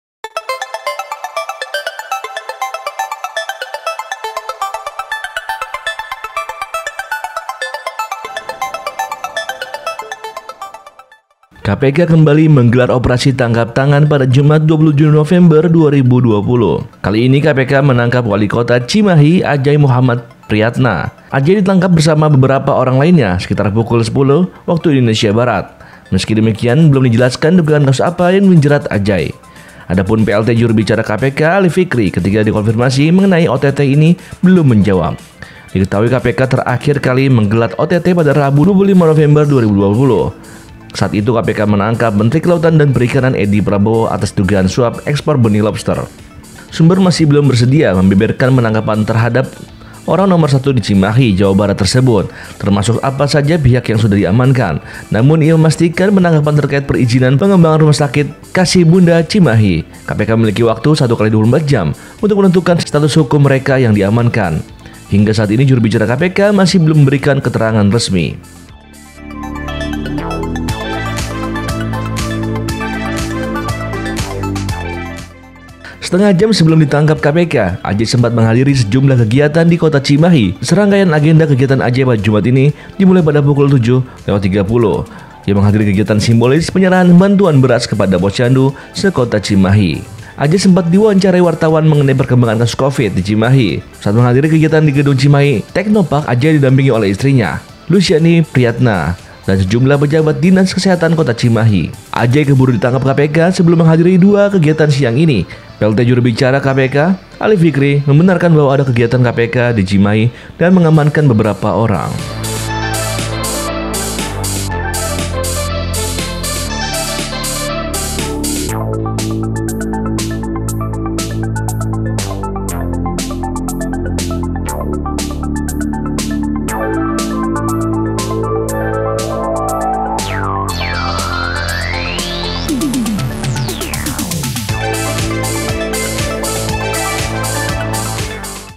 Walkot Cimahi Ajay Di Tangkap KPK
KPK kembali menggelar operasi tangkap tangan pada Jumat (27/11).
Kali ini, KPK menangkap Wali Kota Cimahi, Ajay Muhammad Priatna.
Sumber kumparan menyebut, Ajay ditangkap bersama beberapa orang lainnya sekitar pukul 10.00 WIB.
Meski demikian, belum dijelaskan dugaan kasus apa yang menjerat Ajay.

WALKOT CIMAHI KABARNYA DITANGKAP KPK